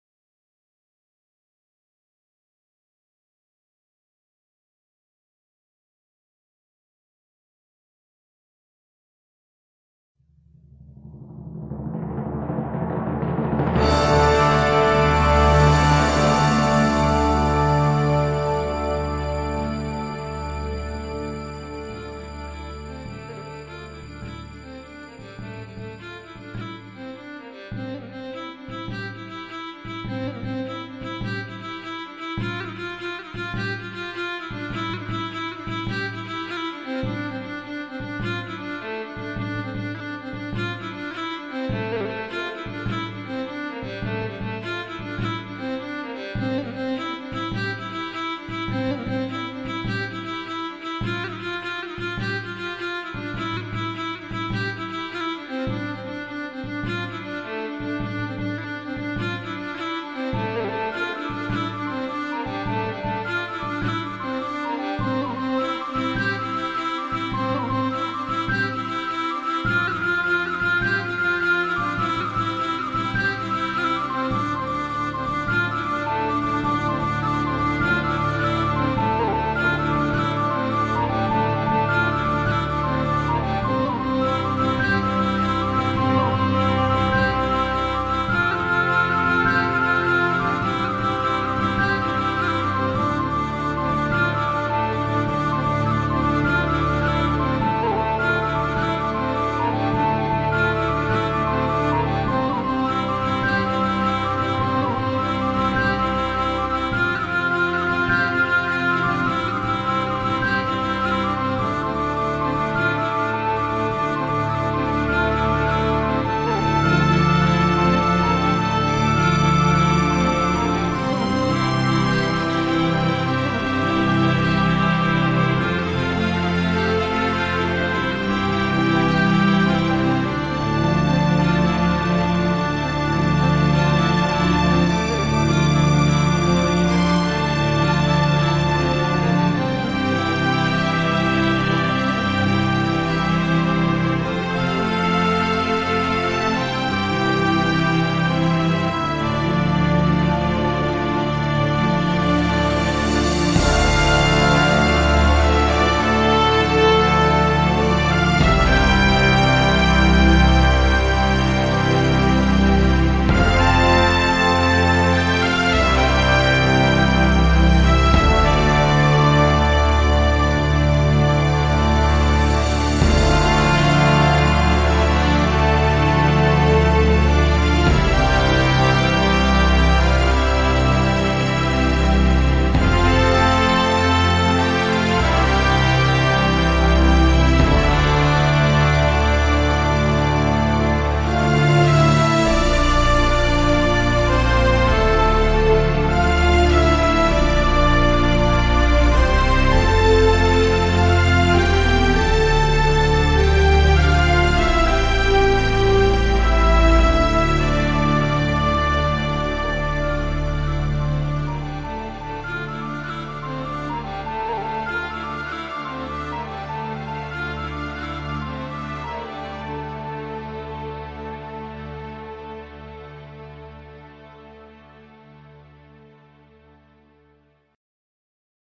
Tradition
Highlights of the Fraternity of Saint Peter priest ordinations.  May 30, 2008.